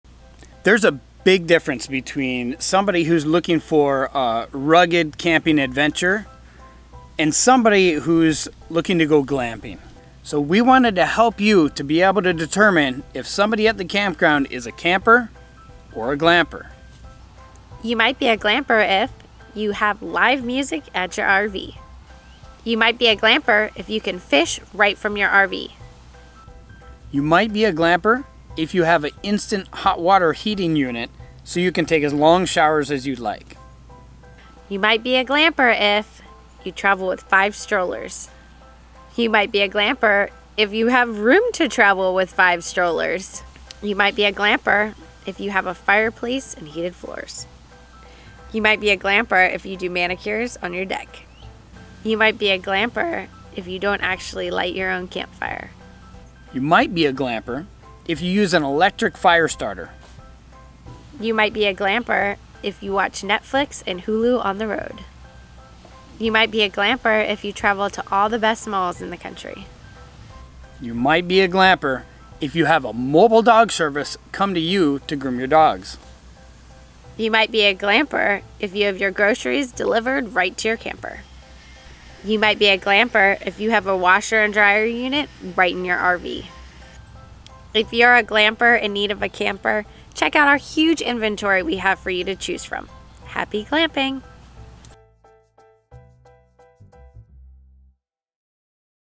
You might be a Glamper if...
Glamping has all of the benefits of traditional camping, but adds in the luxuries of being at home. Are you a glamper?